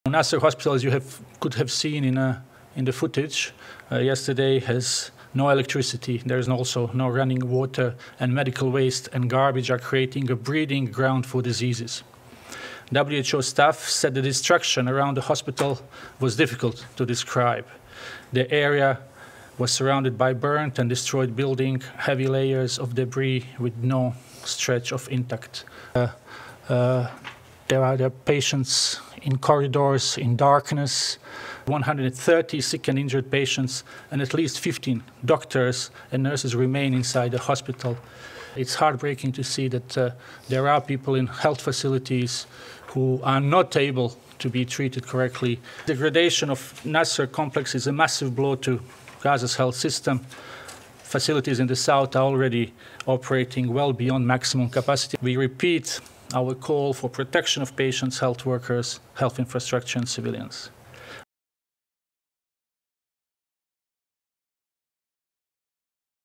Gaza: 32 critical patients transferred from Nasser Hospital - WHO
The World Health Organization (WHO) said on Tuesday that it had completed a second evacuation mission from Gaza's Nasser Hospital, transferring a total of 32 critical patients from the site despite access restrictions, amid ongoing hostilities.

“Together on those two missions, on Sunday and yesterday, there were 32 critical patients, including two children, that have been transferred to two other hospitals in southern Gaza,” said WHO spokesperson Tarik Jasarevic, briefing journalists at the UN in Geneva.

The two life-saving missions were conducted in close partnership with the Palestine Red Crescent Society (PRCS) and the United Nations Office for the Coordination of Humanitarian Affairs (OCHA). The team also provided limited supplies of essential medicines and food for the remaining patients and staff who are otherwise cut off from aid.

“An estimated 130 sick and injured patients, and at least 15 doctors and nurses, remain inside the hospital as the intensive care unit was no longer functioning,” said Mr. Jasarevic. “WHO staff transferred only remaining ICU patients to a different part of the complex where other patients are receiving basic care.”

The transfer of patients was requested by the Nasser hospital staff after the facility became non-functional following a military raid on 14 February, at the end of a week-long siege. Weak and frail patients were transferred amidst active conflict near the aid convoy. Road conditions hindered the swift movement of ambulances, placing the health of patients at further risk.

“No electricity and there is also no running water -- and medical waste and garbage are creating a breeding ground for diseases,” reported WHO’s spokesperson. “WHO staff said the destruction around the hospital was difficult to describe. The area was surrounded by burnt and destroyed building, heavy layers of debris with no stretch of intact roads,” Mr. Jasarevic added.

Efforts to facilitate further patient referrals amidst the ongoing hostilities are ongoing. Prior to the recent evacuation missions, WHO had received two consecutive denials to access the Nasser Medical Complex for medical assessment, causing delays in urgently needed patient referral. At least five patients are reported to have died in the Intensive Care Unit before any missions or transfers were possible.

“Ideally, what we would like to see is to have (the) hospital rebuilt, have health workers be back, have fuel, food and health supplies being brought back to Nasser and other hospitals,” Mr. Jasarevic said. “So a health system can be rebuilt, and then health workers can have a place to work, and then people would have a place to go and get their health services, ideally.”